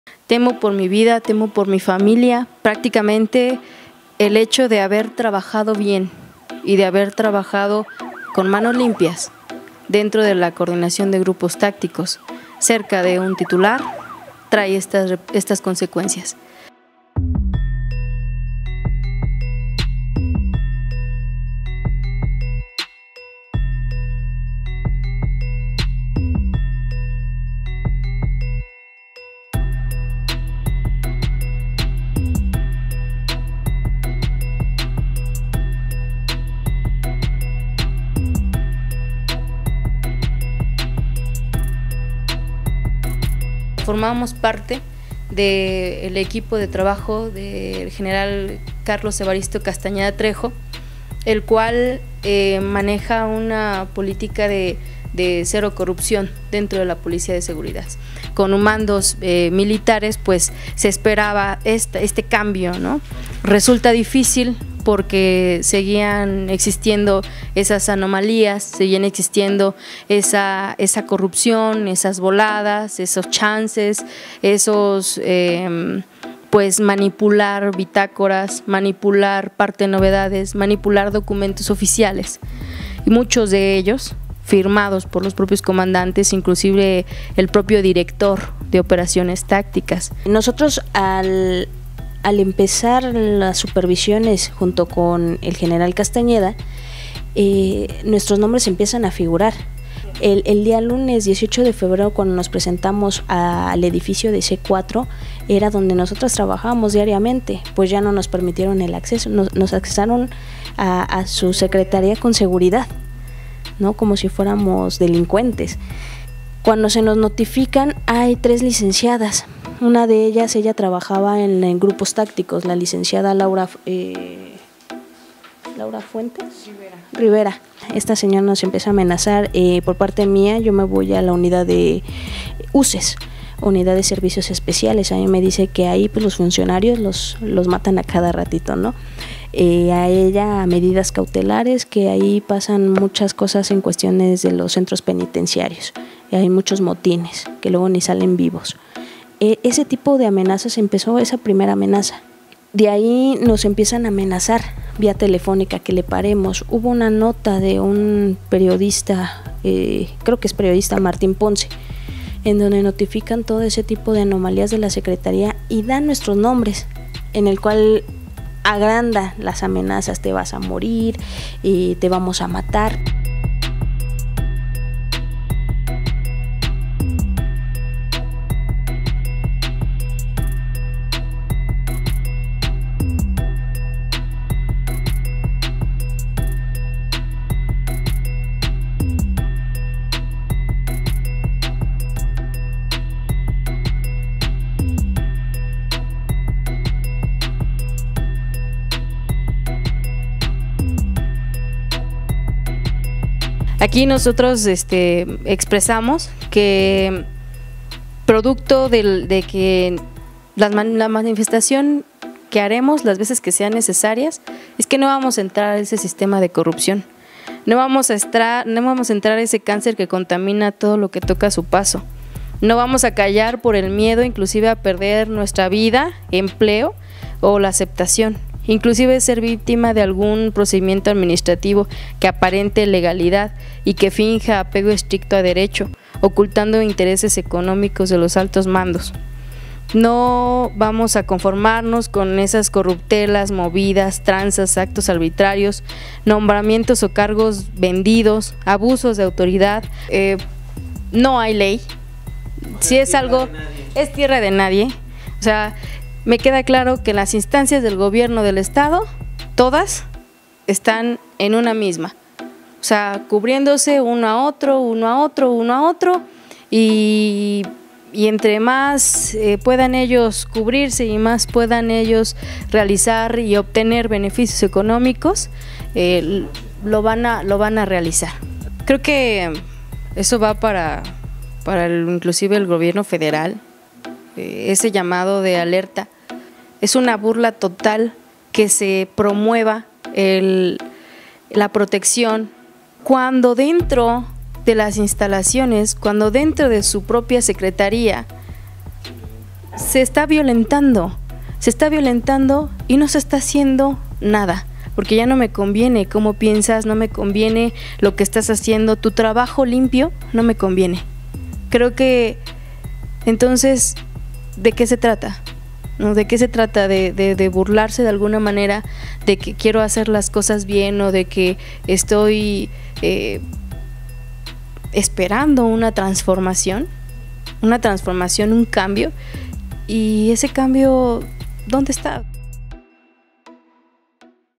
Mandos de la policía denuncian corrupción y directivos los amenazan de muerte
Por denunciar actos de corrupción, nepotismo y una serie de irregularidades al interior del cuerpo de la Policía Especial de Seguridad (FES) del Estado de México, tres mandos sufrieron amenazas de muerte por los directivos de la dependencia.

Un general y dos subalternos del cuerpo de elementos de seguridad de la entidad mexiquense denunciaron a Publimetro acoso laboral, hostigamiento verbal y hasta mensajes de muerte en su contra por revelar inconsistencias en contra de los altos mandos al interior del cuerpo de seguridad. +

En entrevista con Publimetro los tres integrantes de la dependencia mexiquense del Estado del México revelaron que han sido presionados por el subsecretario de la Secretaría de Seguridad Pública del Estado de México, Sergio Hernando Chávez para que presenten su renuncia.

Esto, luego de que se negaran a pagar “diezmo” y denunciaran una serie de inconsistencias al interior de la policía élite de la entidad, a quienes se les señaló de pagar moches para no presentarse a trabajar, estar inmiscuidos en actos de corrupción e incluso sobornar a funcionarios.

Alicia Olvera Aguirre y Jessica Margarita Ortiz González, pertenecientes a la policía de la Fuerza Especial de Seguridad del Estado de México, afirmaron que han sido objeto de represalias en su contra por los directivos de la corporación.

“Nos trataron como delincuentes, tememos por nuestra vida y por nuestra familia. El haber denunciado corrupción nos generó consecuencias para nuestro trabajo y para nuestra propia seguridad”, externaron las servidoras públicas.

Por ello, hicieron un llamado al gobernador, Alfredo del Mazo a pronunciarse sobre este modus operandi y a la titular de la Secretaría de Seguridad del Estado de México, Maribel Cervantes para frenar cualquier tipo de amenazas en su contra y sancionar a los responsables.